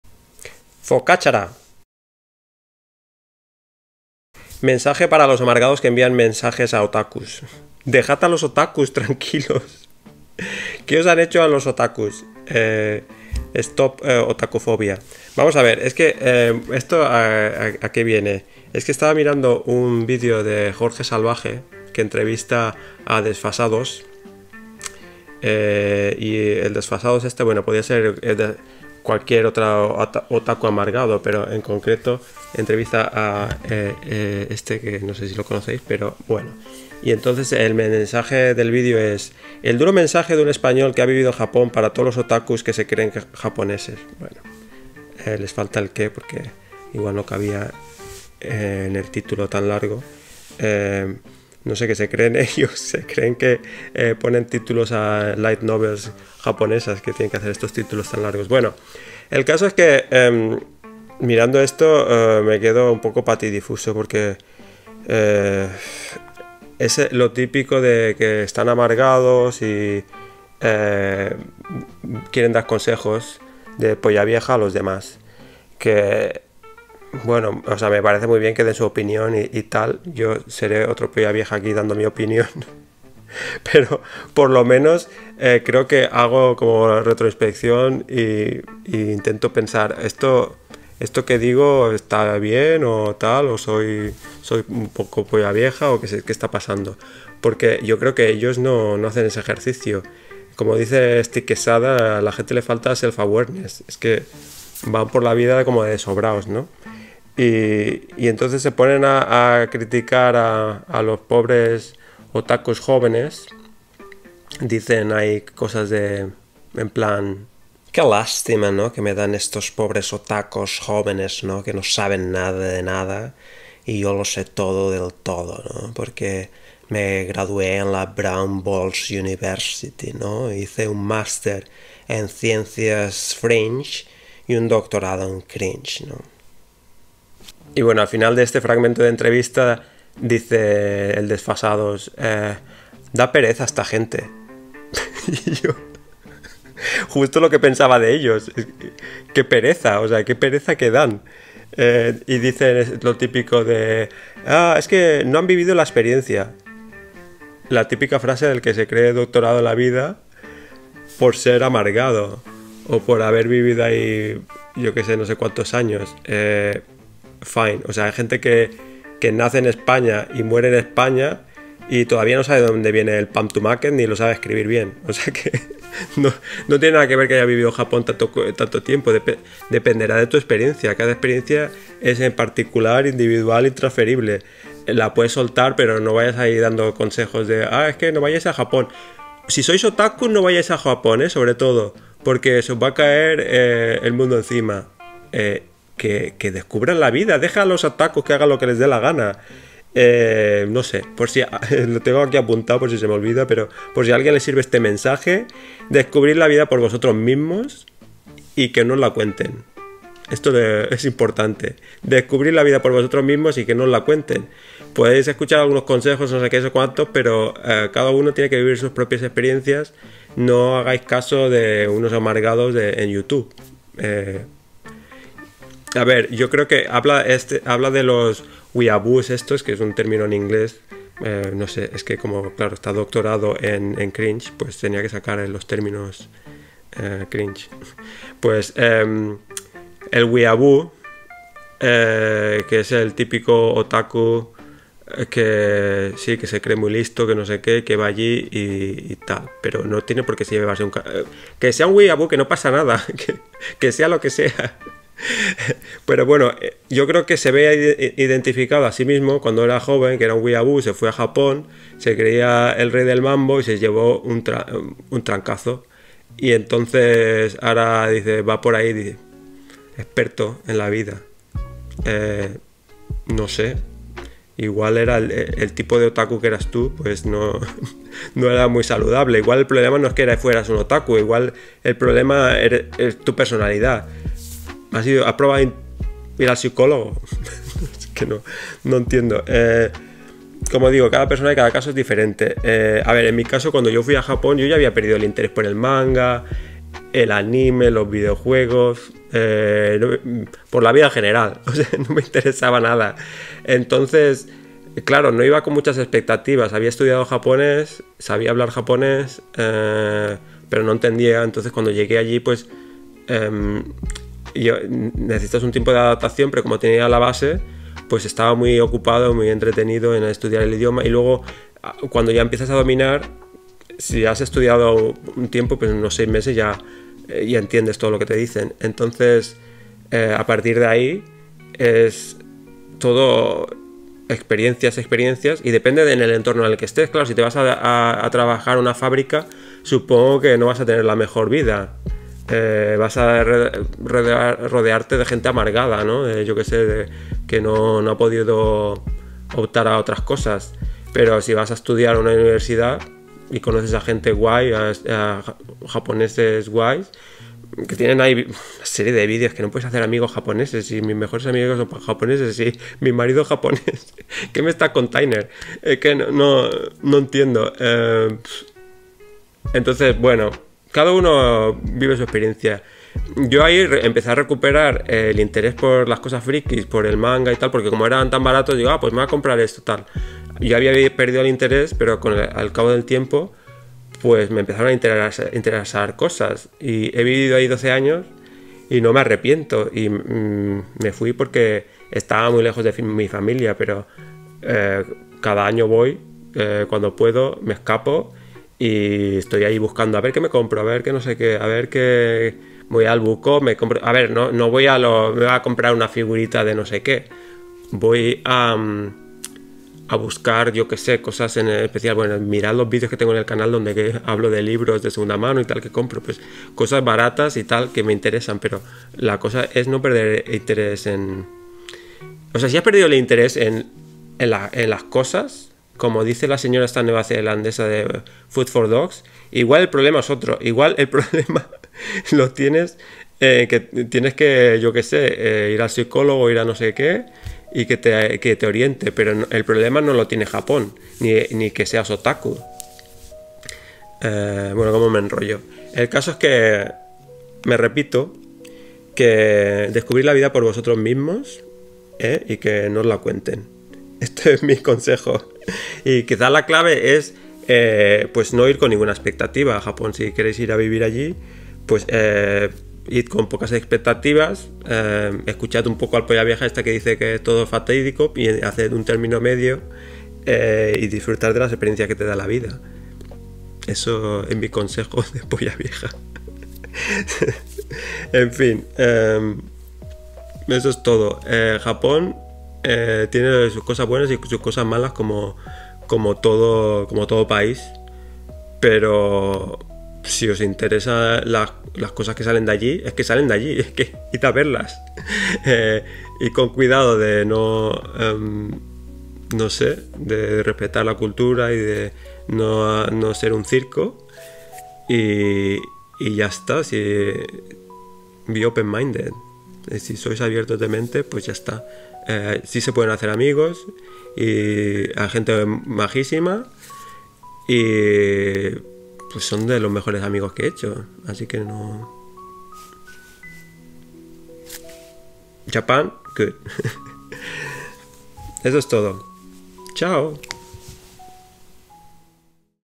Re: duro mensaje de un español que ha vivido en Japón para todos los otakus que se creen japoneses
Respondiendo a un clip de Jorge Salvaje y desFasa2.

Mi consejo:
Descubrid la vida por vosotros mismos y que no os la cuenten.

00:04 Stop otakufobia
01:07 Amargados y pollaviejas
01:58 Mensaje lastímero a otakos
02:17 Da pereza esta gente
03:09 No vayáis a Japón
03:24 Mi mensaje
03:47 Weeaboo
04:51 Mi caso en Japón
05:57 No se pueden hacer amigos en Japón
06:35 Intereses frikis
07:26 Id sin expectativas